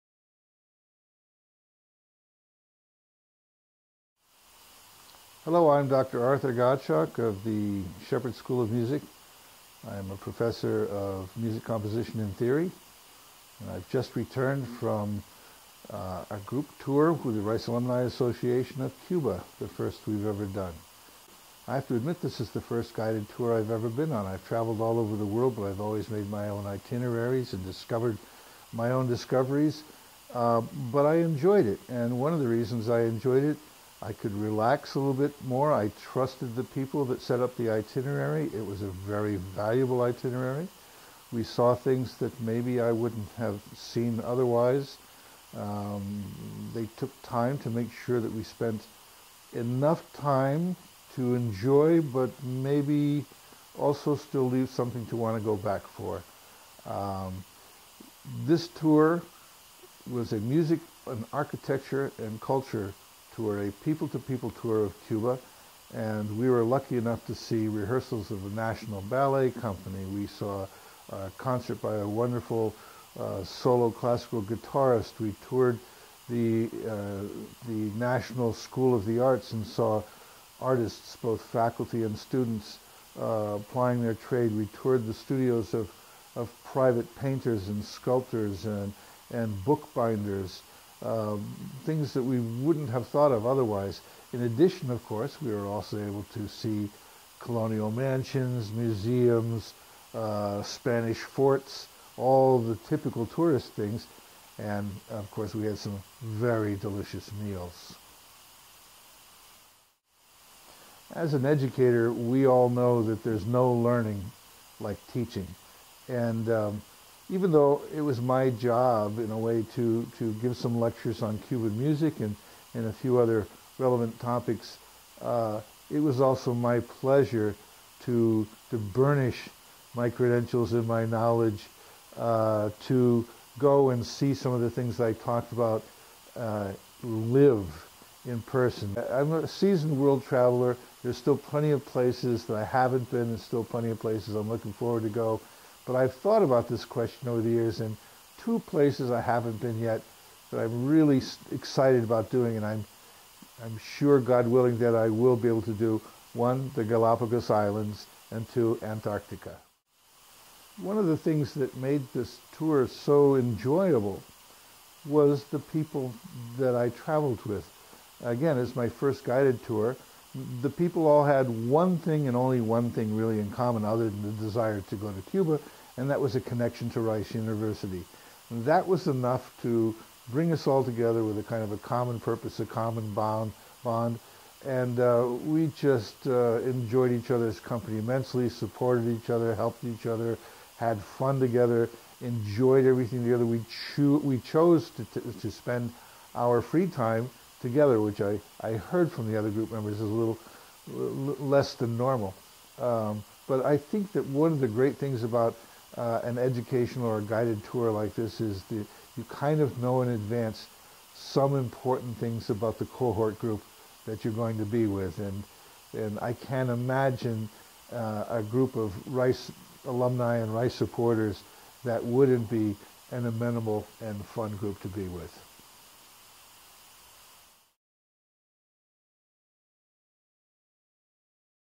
Faculty Spotlight: Dr. Art Gottschalk
Dr. Art Gottschalk is a professor of Music Composition in the Shepherd School of Music at Rice University, and has also served as a lecturer for the Rice Alumni Traveling Owls trips to Cuba in the spring of 2013. Here he discusses some of the rewards of educational trips with the Traveling Owls program, as well as detailing some of his own rewarding experiences abroad.